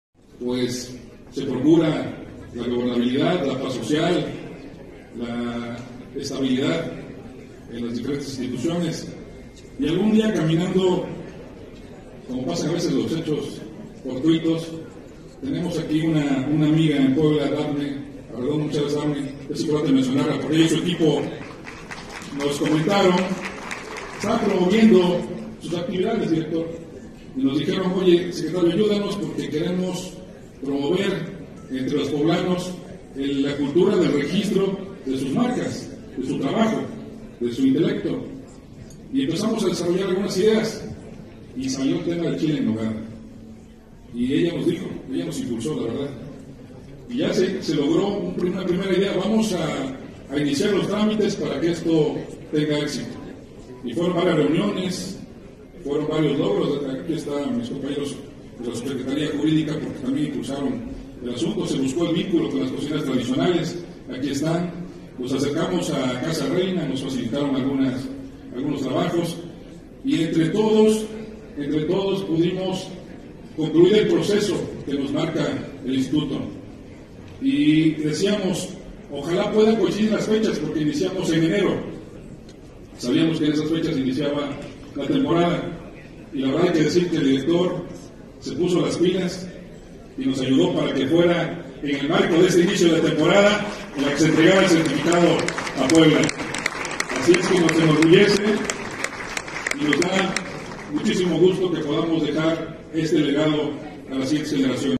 Gobierno de Puebla buscará registro de esferas de Chignahuapan ante el IMPI
El secretario de Gobernación Estatal, Javier Aquino, destacó que buscarán el registro ante el IMPI de las esferas hechas en el municipio de Chignahuapan y los bordados poblanos, al igual que lo lograron con el Chile en Nogada.